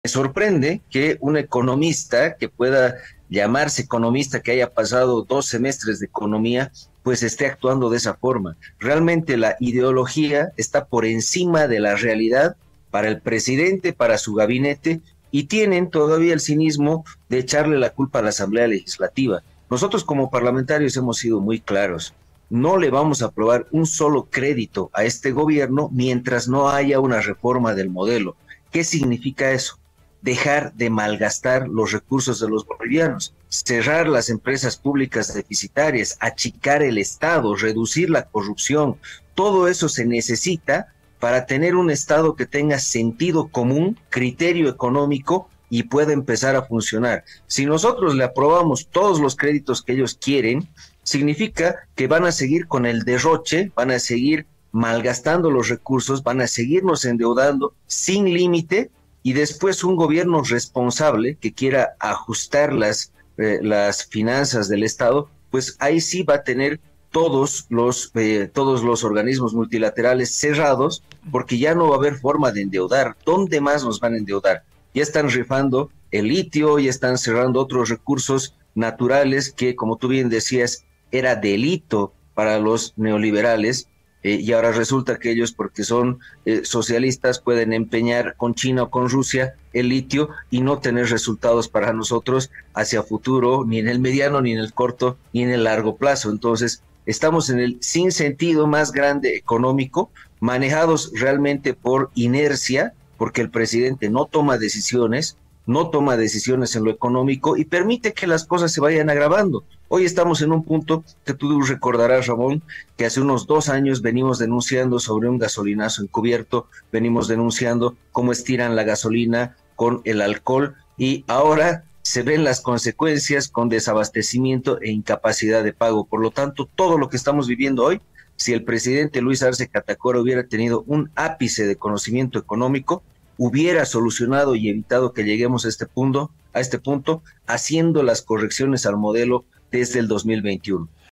Diputado Alejandro Reyes: El Presidente maneja la economía en base a ideología
El diputado Alejandro Reyes sostuvo que el presidente LuisArce basa el manejo de la economía del país “por encima de la realidad y en el marco de su ideología” y reiteró que desde el Parlamento no aprobarán créditos porque el próximo Gobierno no podrá ajustar la estructura económica por la cantidad de deudas.